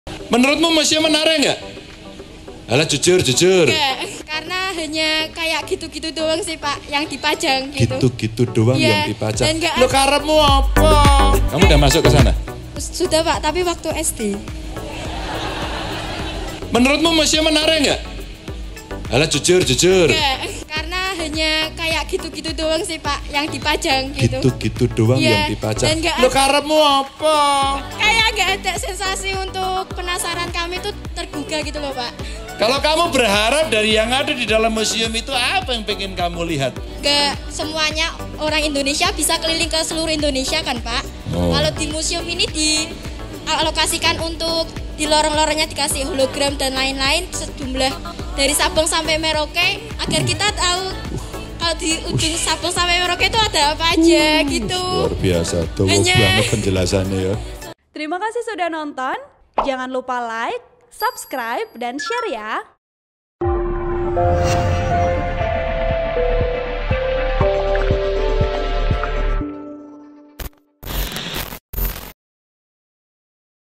Momen Lucu Pelajar Beri Saran ke Ganjar Pranowo soal Pengelolaan Museum yang Menarik
Menurut pengakuan pelajar tersebut, museum kurang menarik.

"Gitu-gitu doang pak, yang dipajang," ujar pelajar.

Reaksi lucu Ganjar Pranowo menanggapi pernyataan pelajar tersebut pun mengundang gelak tawa.